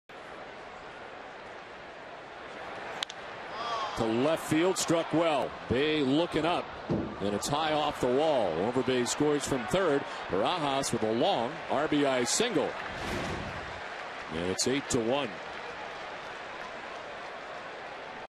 2009/05/20 Barajas' RBI single
TOR@BOS: Barajas singles to left to bring in a run